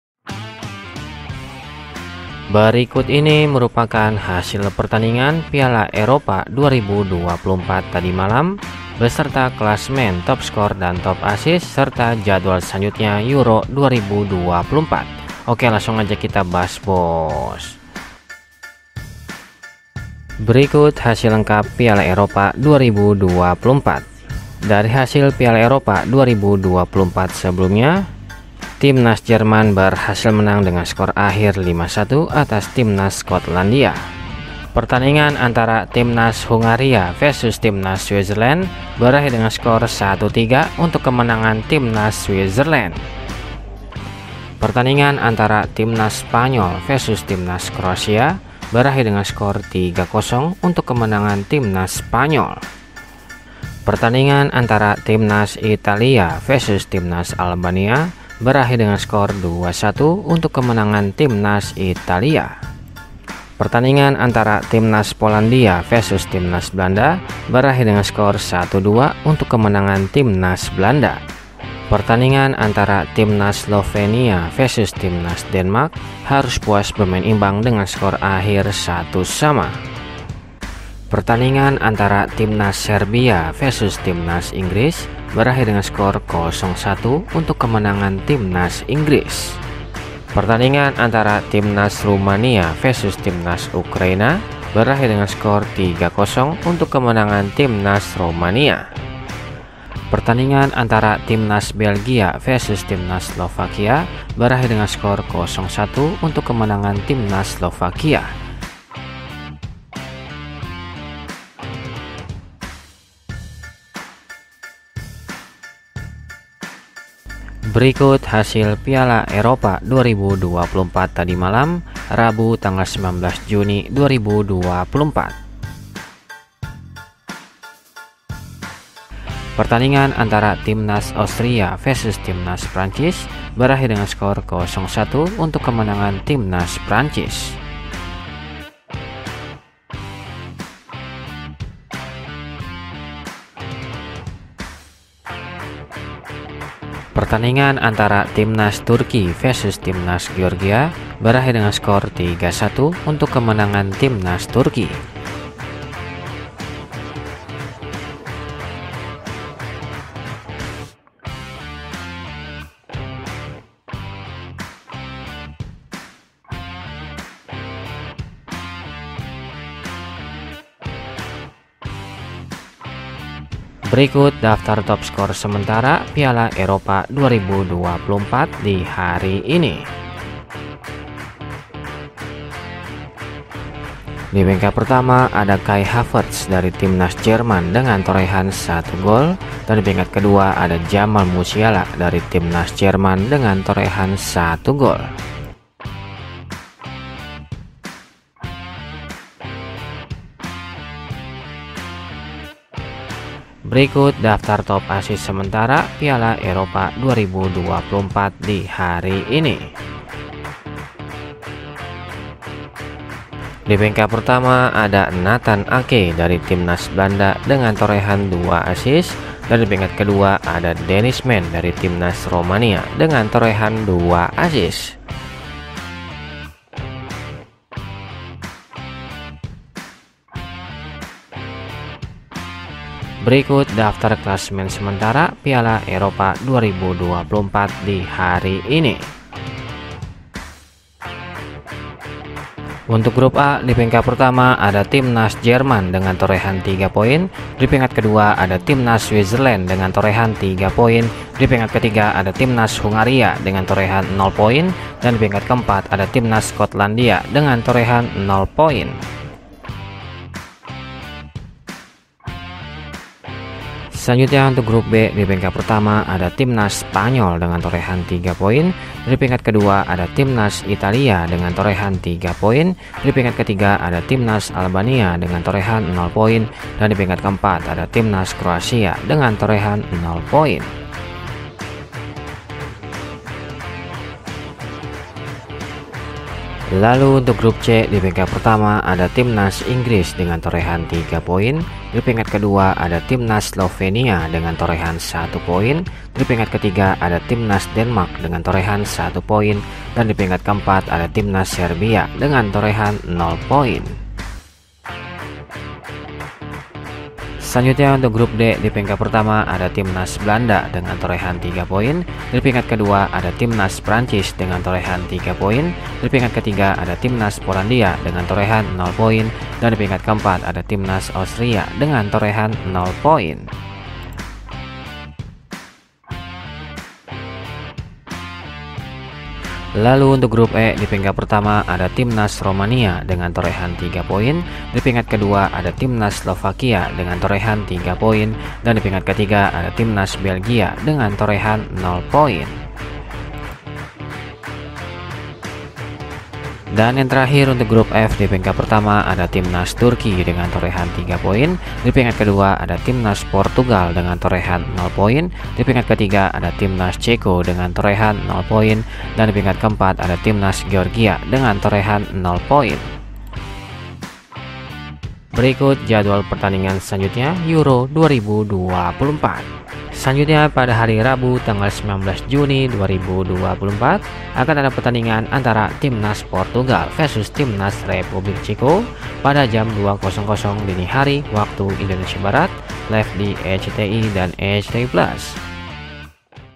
Hasil Piala Eropa 2024 - Turki vs Georgia - Klasemen Piala Eropa 2024 Terbaru - UEFA EURO 2024
Hasil Piala Eropa 2024 Tadi Malam - Turki vs Georgia - Klasemen Piala Eropa 2024 Terbaru Hari Ini - UEFA EURO 2024

inilah hasil lengkap pertandingan Piala Eropa 2024, Rabu 19 Juni 2024

Berikut hasil Piala Eropa terbaru hari ini
rabu, 19 juni 2024
hasil turki vs georgia 3-1
hasil austria vs perancis 0-1